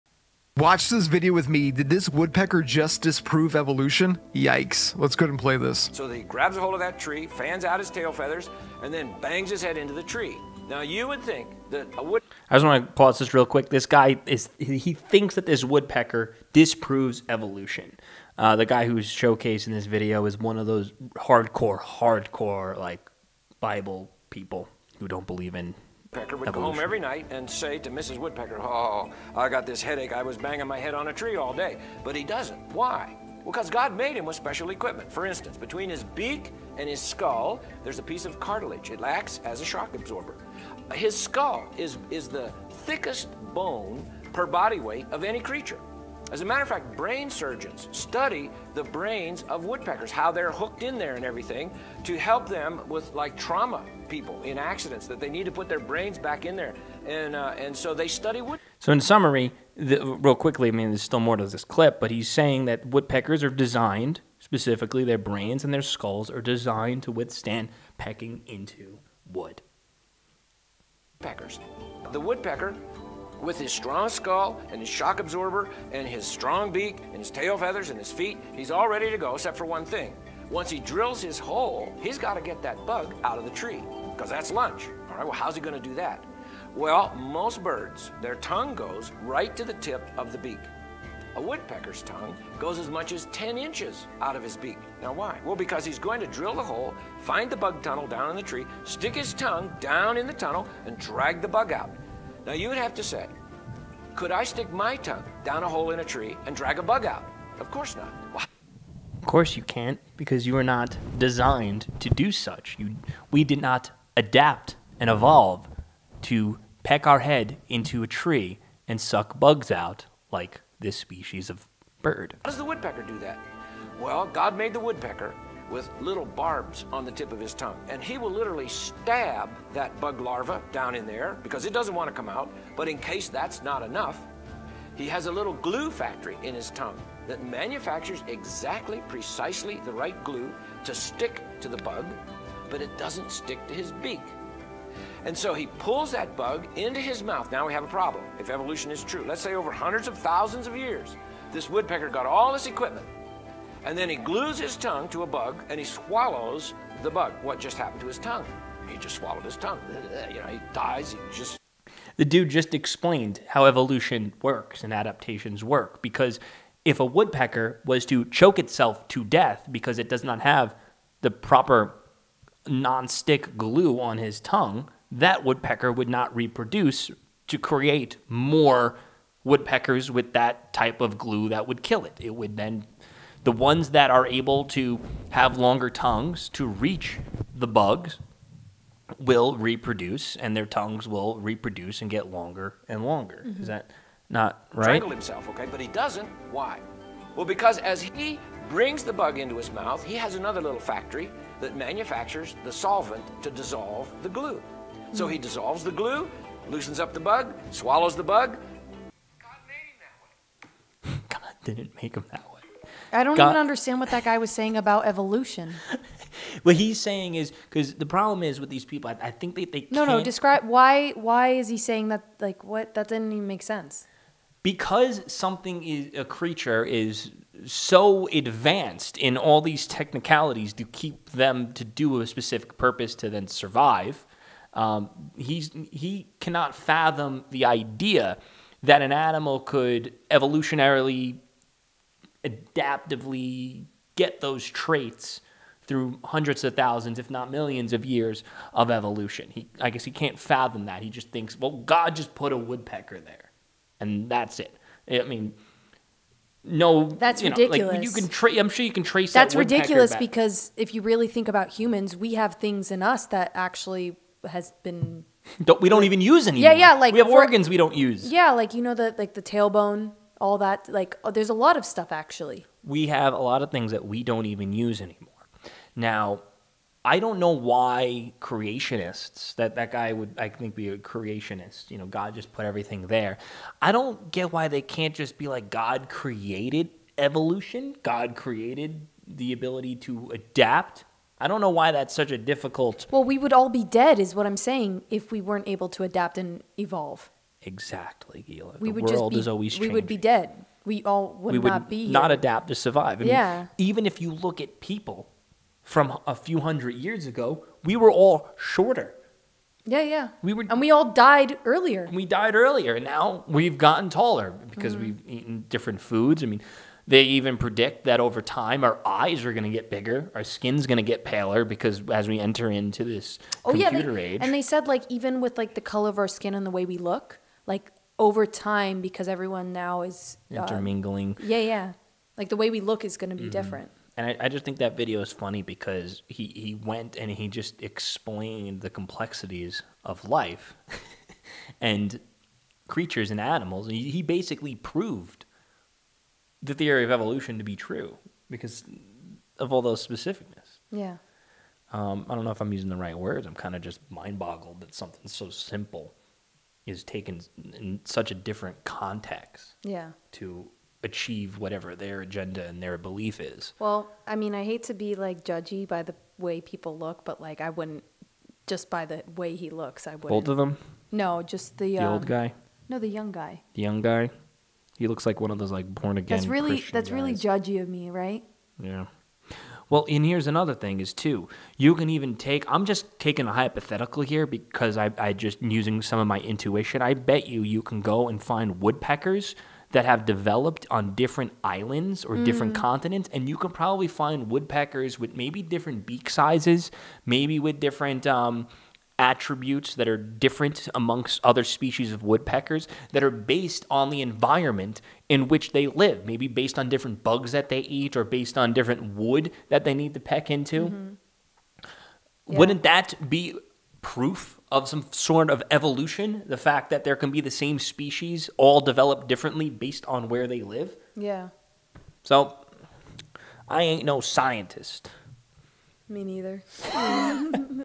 Creationist Accidentally PROVES Evolution is Real
In this shocking video, a creationist accidentally proves the reality of evolution. Watch as we break down the evidence and explain how this unexpected discovery supports the theory of evolution. This is a must-watch for anyone interested in the ongoing debate between creationism and evolution!